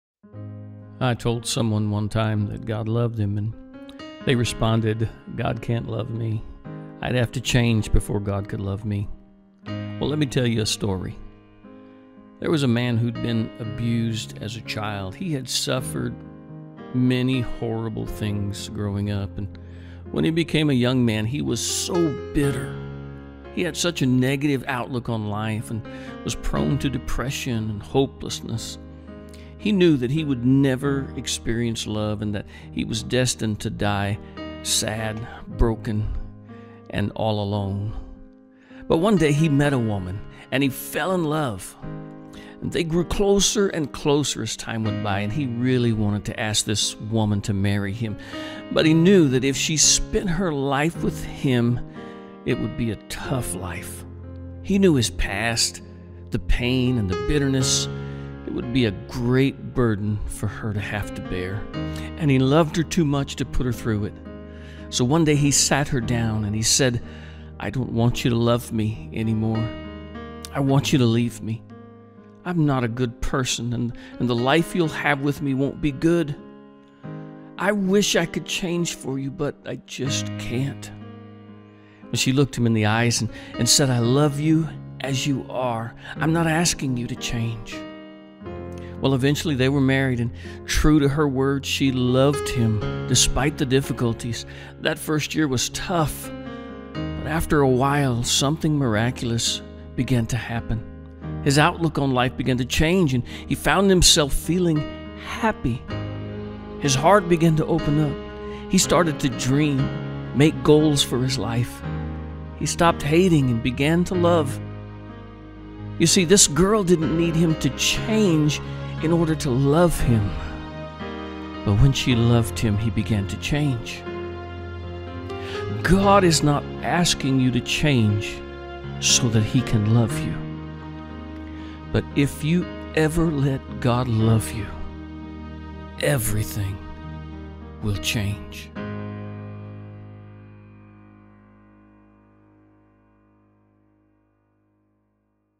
Do you think you have to change before God will love you?
God is not asking you to change before He loves you. He loves you as you are, but His love won't leave you as you are. If you let God love you His love will transform your life.